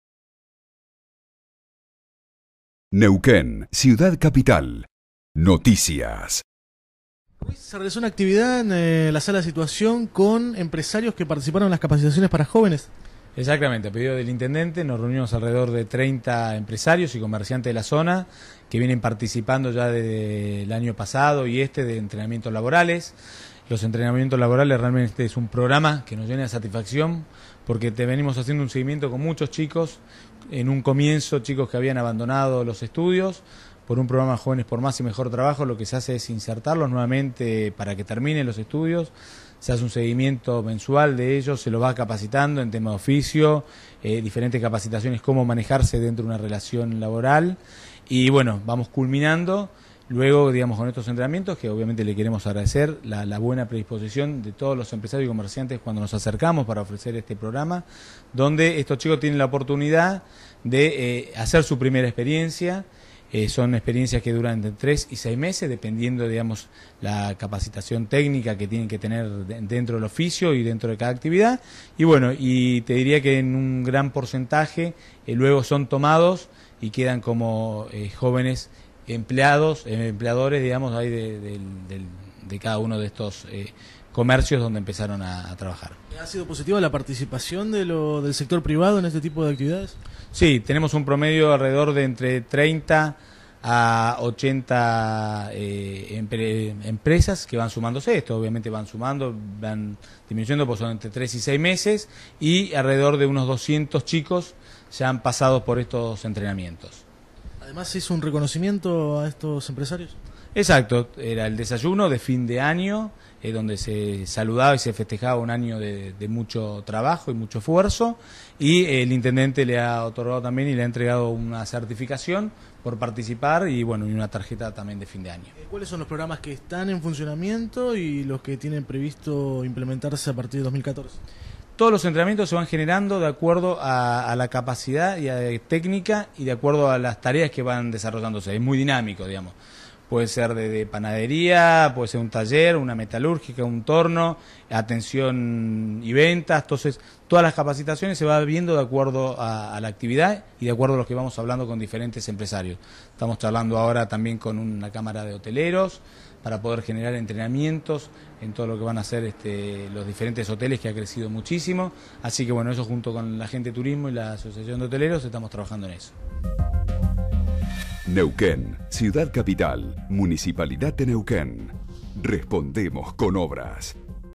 Horacio "Pechi" Quiroga - Reconocimiento a empresarios que capacitaron a jóvenes.
La capacitación se dio en el marco del programa Más y Mejor Trabajo.